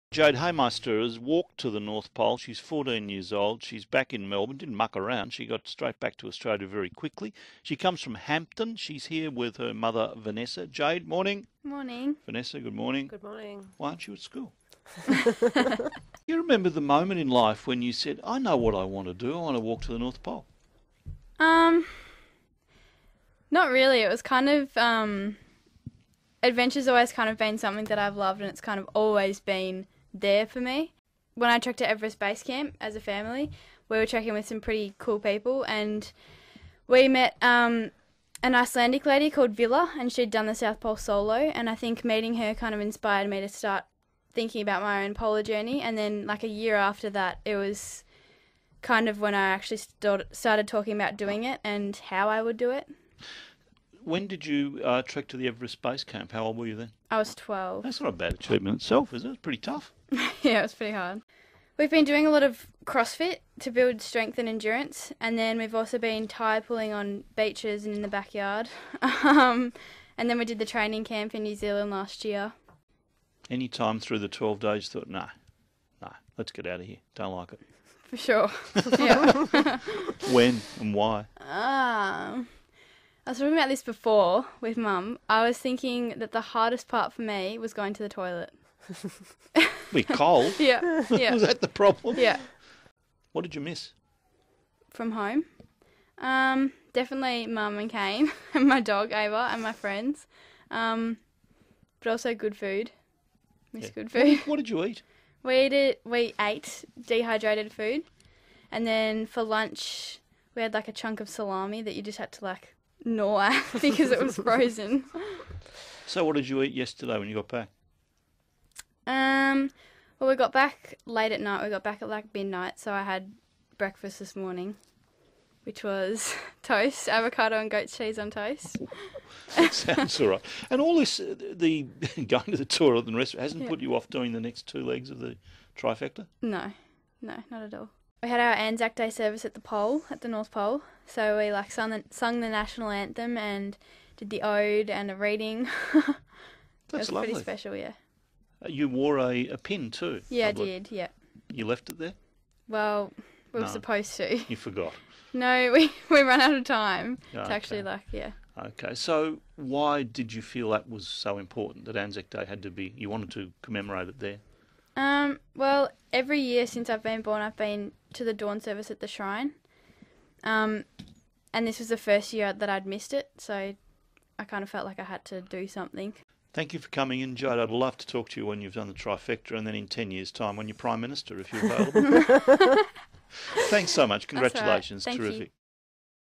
Jade Hameister, the youngest person to make the trek to the North Pole
Jade Hameister joins Neil Mitchell in the studio to speak about her adventures.